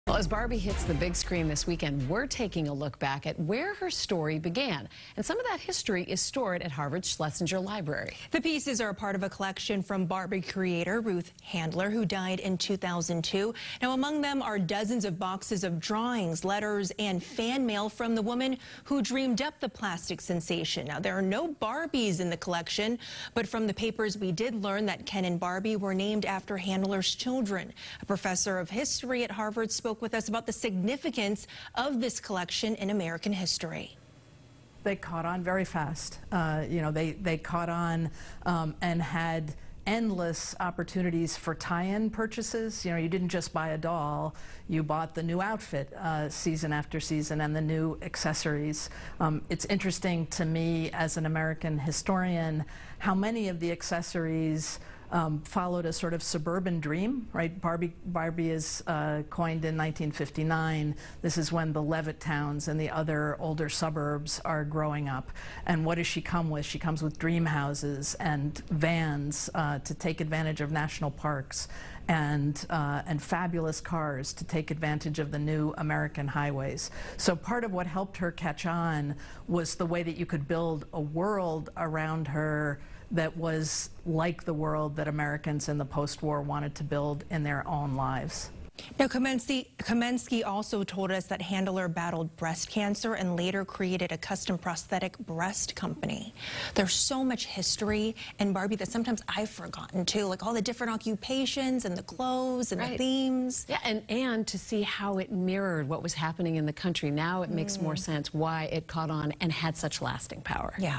A peek into Barbie's past at Harvard University
As the Barbie movie hits the big screen Friday, Harvard University gave WBZ TV a look back at where her story began.